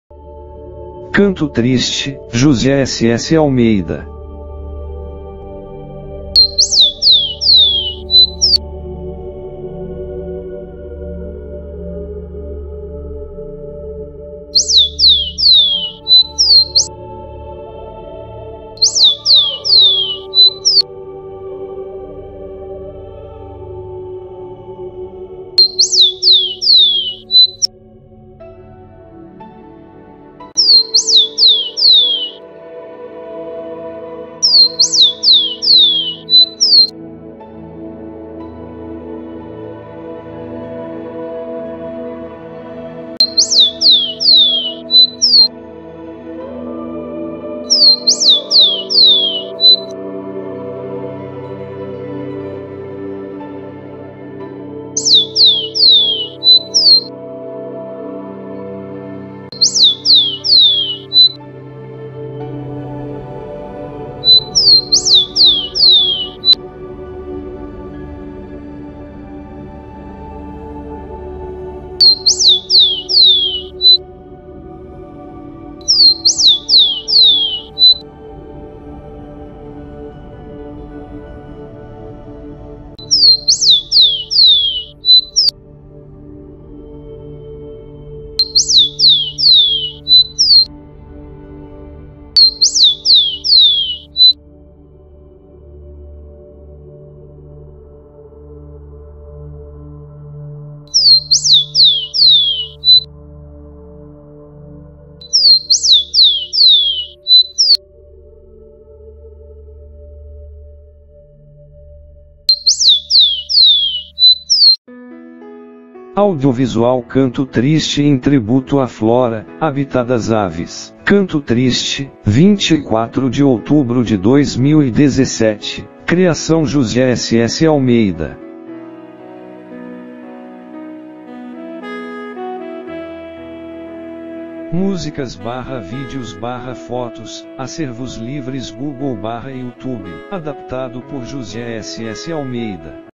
Canto Triste!
Audiovisual Canto Triste em tributo
à Flora - habitat das aves
24 de Outubro de 2017
Criação José SS Almeida